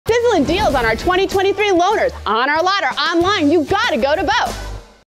Sizzlin Prices at Bo Beuckman Quality Ford
Sizzlin prices on low mileage Service Loaners. Full factory warranty. Eligible for applicable rebates and always Bo's Discount.